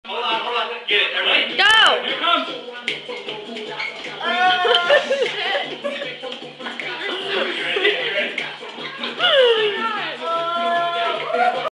crazies
everyone dancing real dirty mexican to sarahs favorite song.